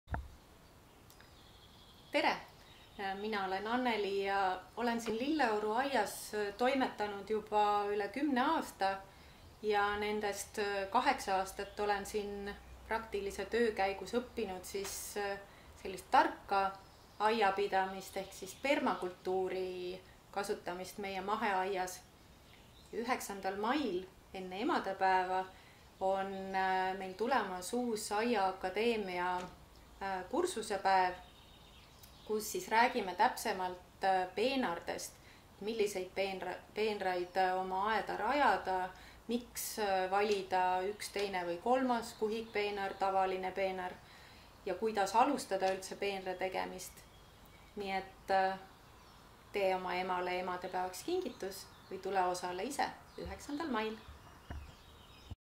Aiaakadeemia: peenrad. Online, 9. mail 2020
Töötoa märksõnad: kuidas peenraid rajada ja miks teha kuhikpeenar või peenrakast. Erinevad võimalused peenardeks vastavalt pinnasele. Mulla kasvatamine ja multšimine.